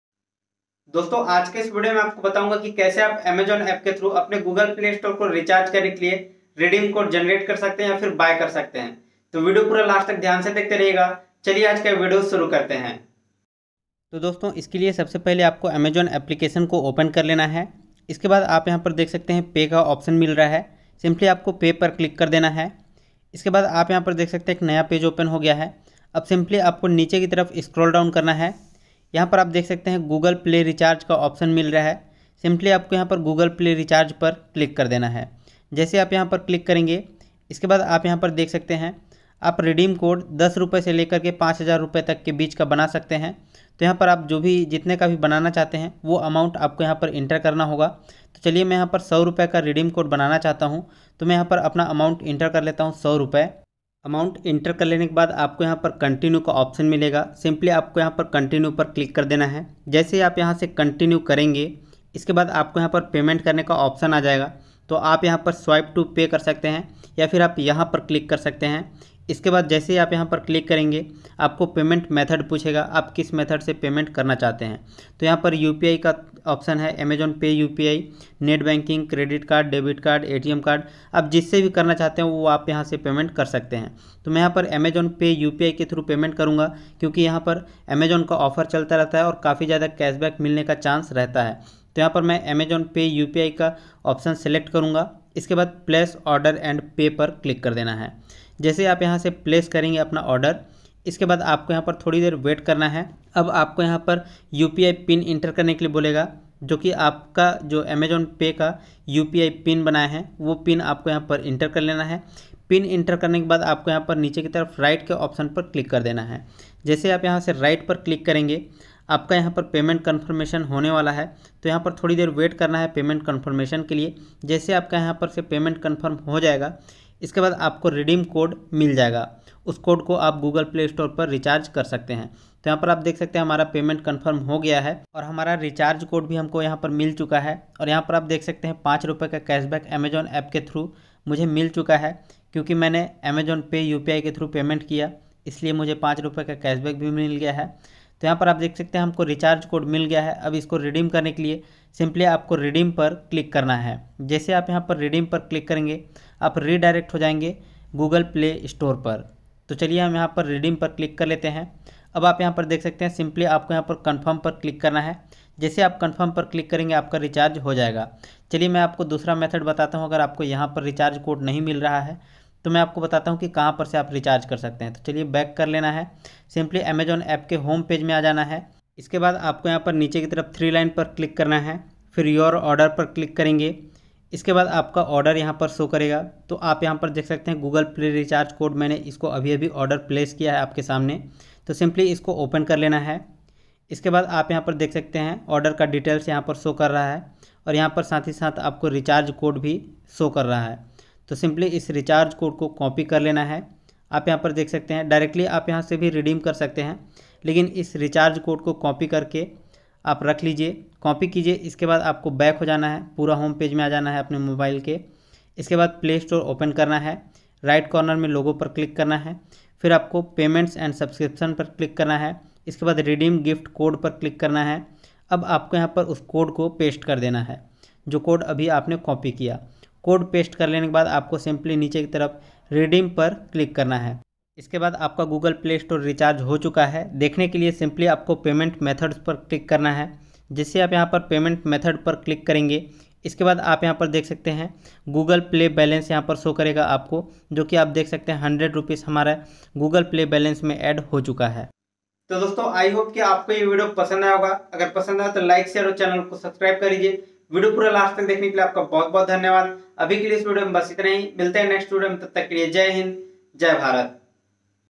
Amazon se Redeem code kaise banaye | play store recharge code kaise banaye
title:

Amazon se Redeem code kaise banaye | play store recharge code kaise banaye | redeem code play store new offer 2021 full process step by step by sunil sahu in hindi.

App Discrimination :

Hey, I just won some amazing scratch cards on Amazon Pay! You can too. Try your luck now.